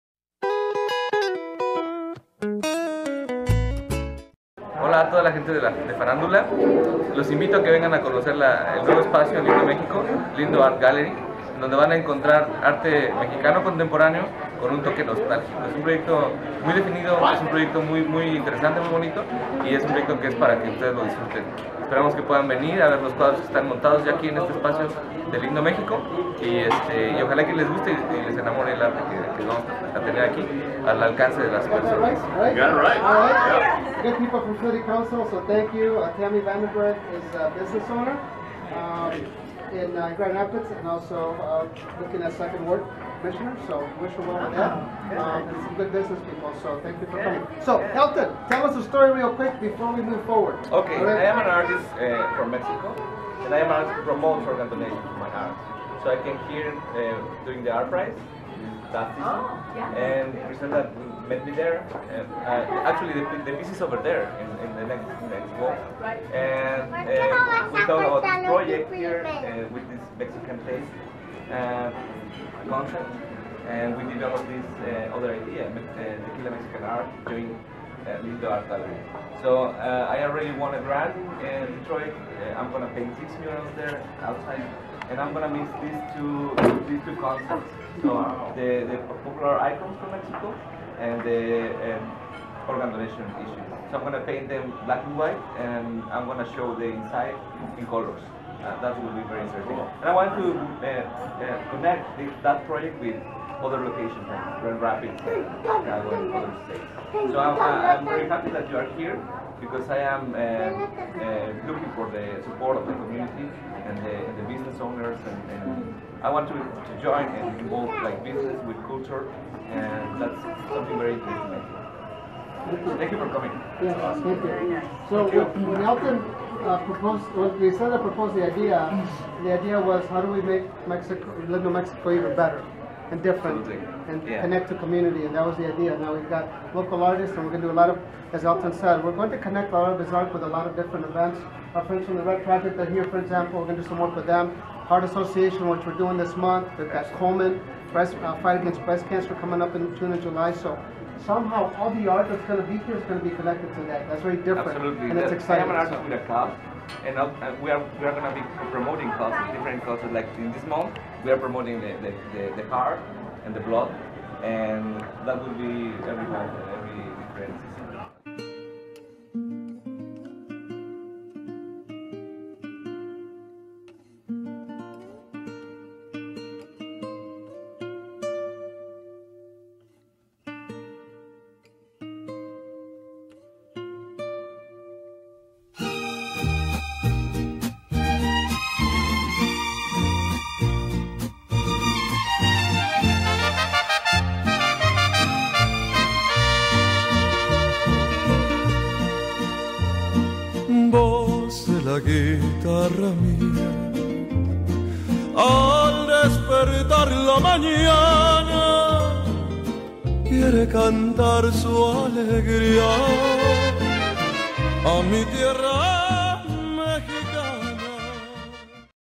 Lindo Art Gallery Big Opening
Lindo Mexico Restaurant opens their Art Gallery to showcase Elton Monroy's Artwork along with other artists.